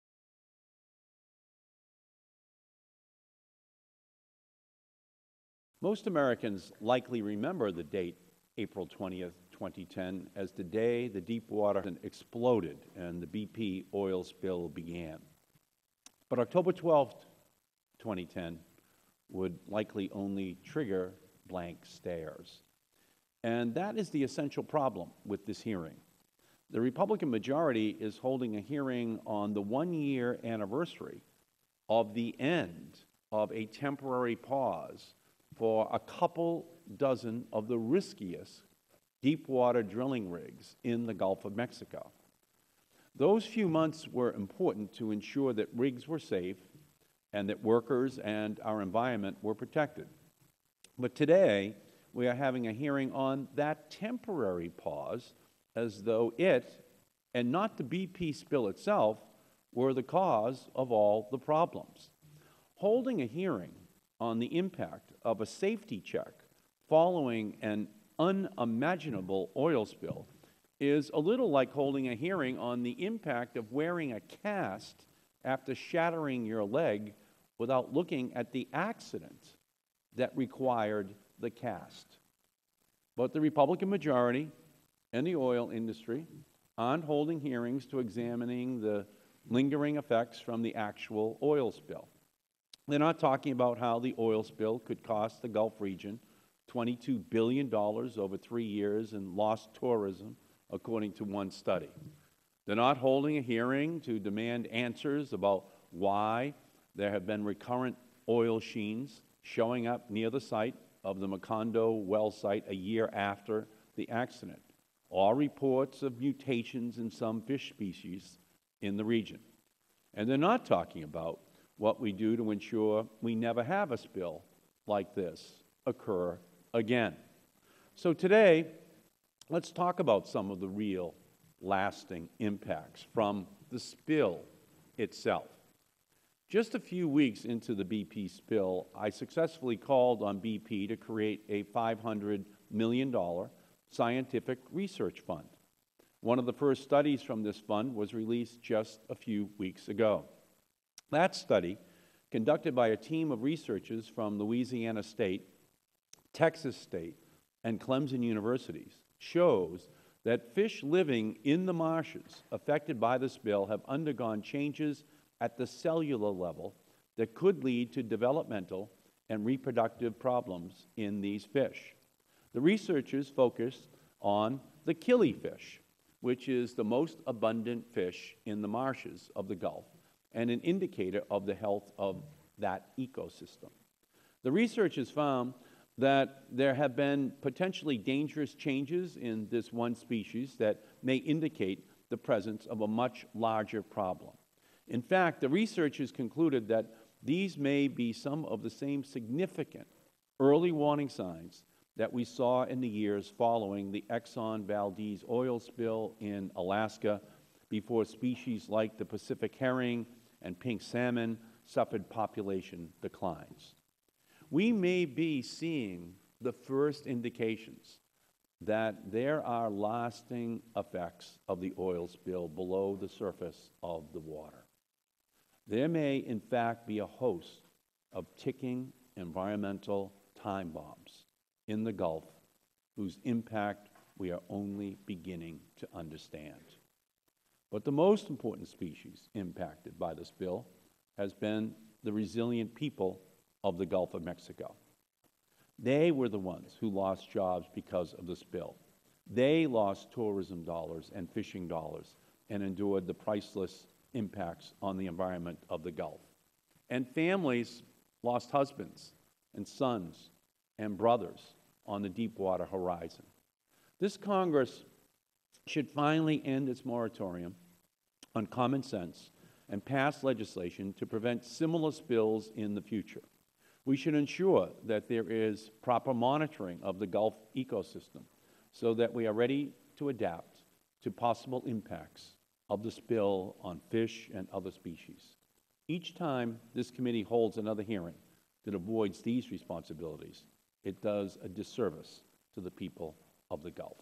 The real, lasting impacts of the BP Oil Spill
Opening Statement of Rep. Edward J. Markey
Natural Resources Committee Oversight Hearing
""One Year after President Obama's Gulf of Mexico 6-Month Moratorium Officially Lifted: Examining the Lingering Impacts on Jobs, Energy Production and Local Economies."

Most Americans likely remember the date April 20, 2010 as the day the Deepwater Horizon exploded and the BP oil spill began.

But October 12, 2010 would likely only trigger blank stares.

And that is the essential problem with this hearing. The Republican Majority is holding a hearing on the one year anniversary of the end of a temporary pause for a couple dozen of the riskiest deepwater drilling rigs in the Gulf of Mexico.

Those few months were important to ensure that rigs were safe and that workers and our environment were protected. But today we are having a hearing on that temporary pause as though it, and not the BP spill itself, were the cause of all problems.

Holding a hearing on the impact of a safety check following an unimaginable oil spill is a little like holding a hearing on the impact of wearing a cast after shattering your leg, without looking at the accident that required the cast.

But the Republican Majority and the oil industry aren't holding hearings to examine the lingering effects from the actual oil spill.

They're not talking about how the oil spill could cost the Gulf region $22.7 billion over three years in lost tourism, according to one study.

They're not holding a hearing to demand answers about why there have been recurrent oil sheens showing up near the site of the Macondo well site a year after the accident, or reports of mutations in some fish species in the region.

And they're not talking about what we do to ensure we never have a spill like this again.

So today, let's talk about some of the real lasting impacts from the spill itself.

Just a few weeks into the BP spill, I successfully called on BP to create a $500 million scientific research fund. One of the first studies from this fund was released just a few weeks ago.

That study, conducted by a team of researchers from Louisiana State, Texas State and Clemson Universities, shows that fish living in the marshes affected by the spill have undergone changes at the cellular level that could lead to developmental and reproductive problems in these fish. The researchers focused on the killifish [KILL-EE-FISH], which is the most abundant fish in the marshes of the Gulf and an indicator of the health of that ecosystem.

The researchers found that there have been potentially dangerous changes in this one species that may indicate the presence of a much larger problem. In fact, the researchers concluded that these may be some of the same significant early warning signs that we saw in the years following the Exxon Valdez oil spill in Alaska before species like the pacific herring and pink salmon suffered population declines. We may be seeing the first indications that there are lasting effects of the oil spill below the surface of the water. There may in fact be a host of ticking environmental time bombs in the Gulf whose impacts we are only beginning to understand.

But the most important species impacted by the spill has been the resilient people of the Gulf of Mexico. They were the ones who lost jobs because of the spill. They lost tourism dollars and fishing dollars and endured the priceless impacts on the environment of the Gulf.

And families lost husbands, sons and brothers on the Deepwater Horizon.

This Congress should finally end its moratorium on common sense and pass legislation to prevent similar spills in the future. We should ensure that there is proper monitoring of the Gulf ecosystem so that we are ready to adapt to possible impacts of the spill on fish and other species. Each time this Committee holds another hearing that avoids these responsibilities, it does a disservice to the people of the Gulf.